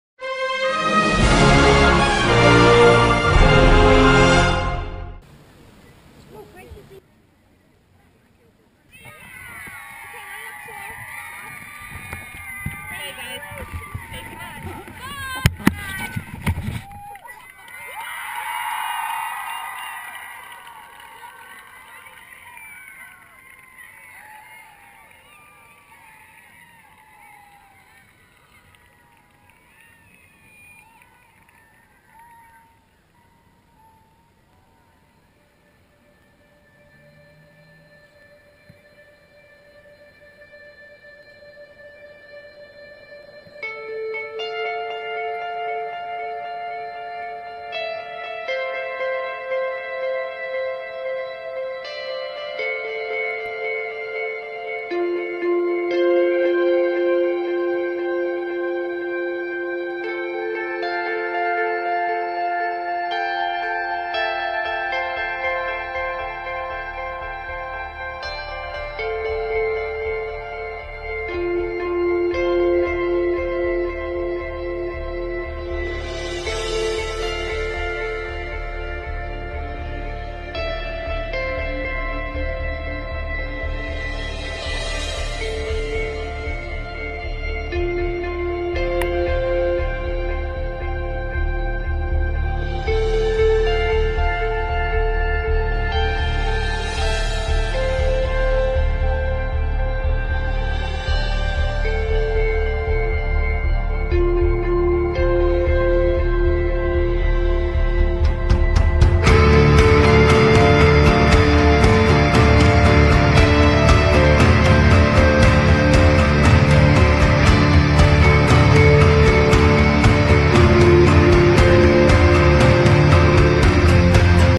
Eagle in Space: Weather Balloon Launch
After waiting for 7 months Project Icarus was finally found. It used a 6ft diameter weather balloon with a GoPro camera. This is the last chapter of Project Icarus